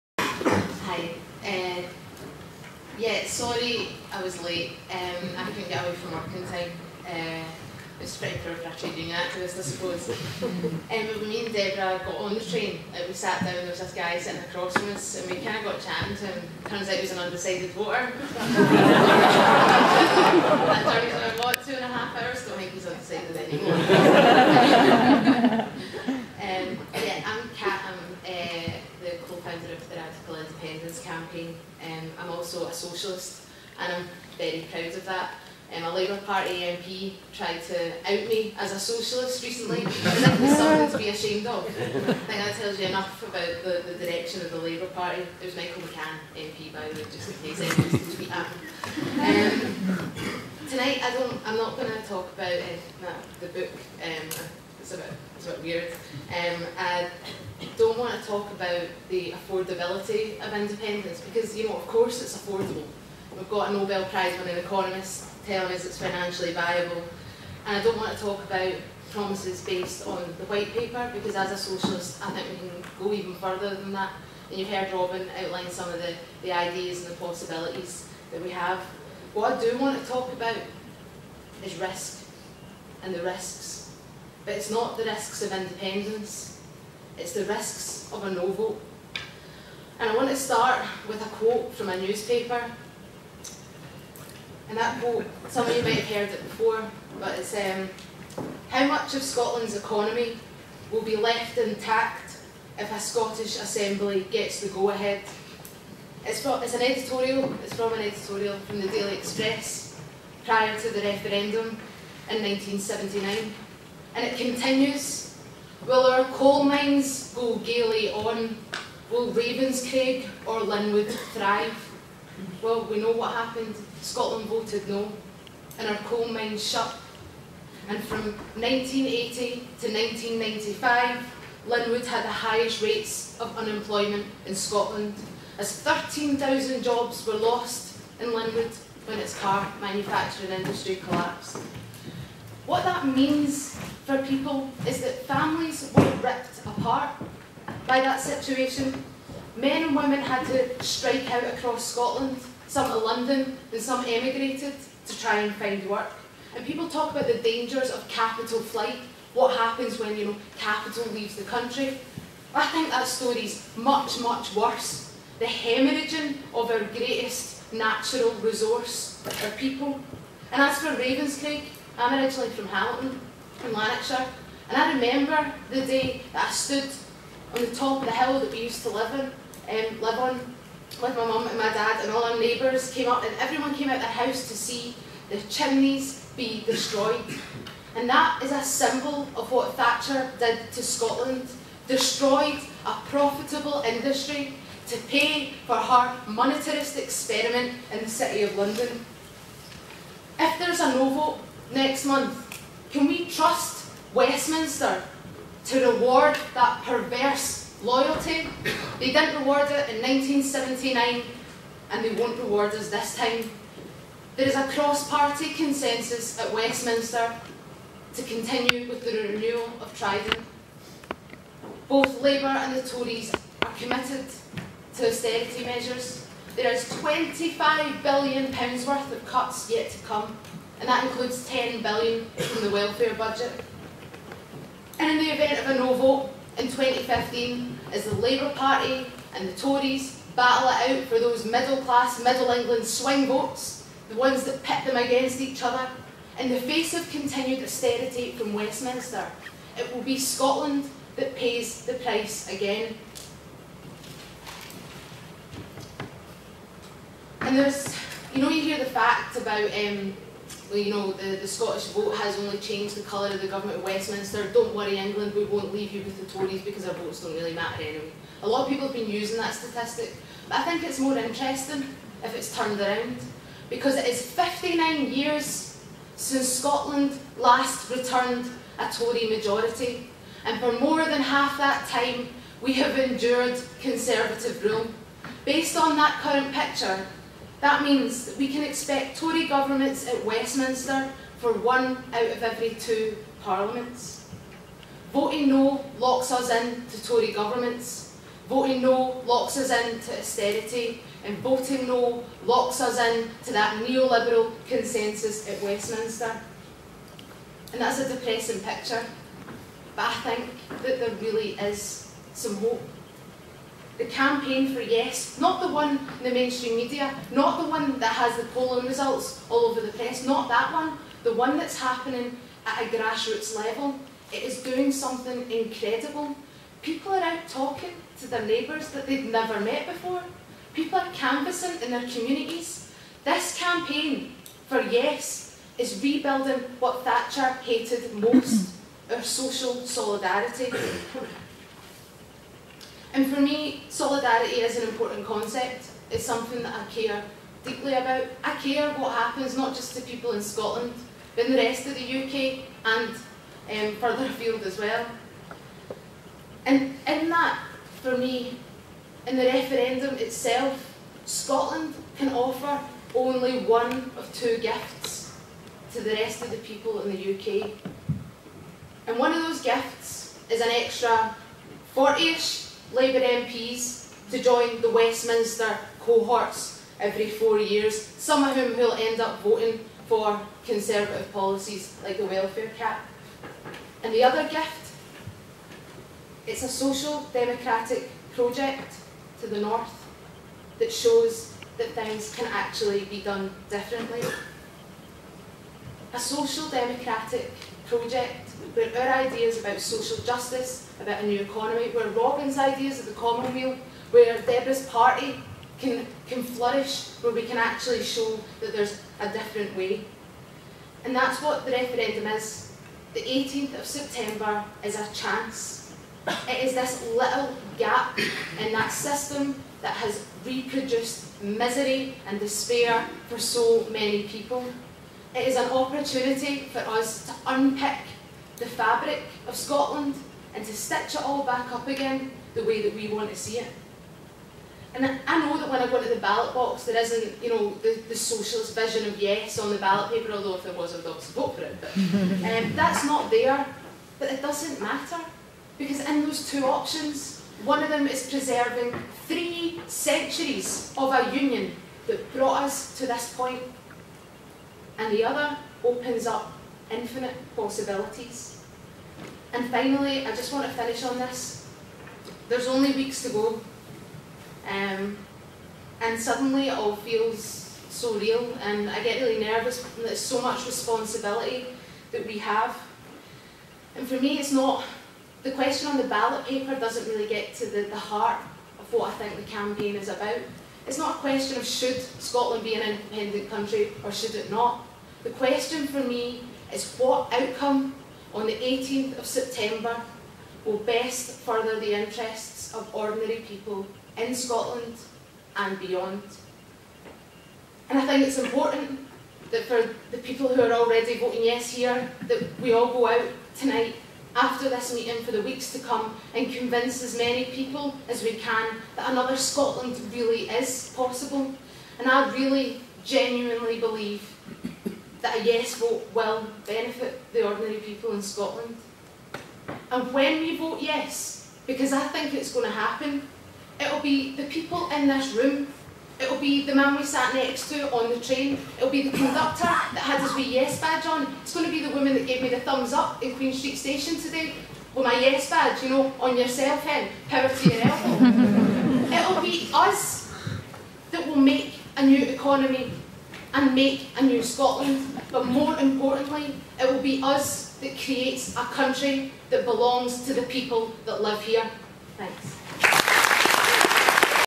Cat Boyd from Radical Independence, speaking at Yes Aberdeen meeting, 28th August 2014
Cat Boyd from Radical Independence speaking at Yes Aberdeen meeting, 28th August 2014, MacRobert Building, University Of Aberdeen.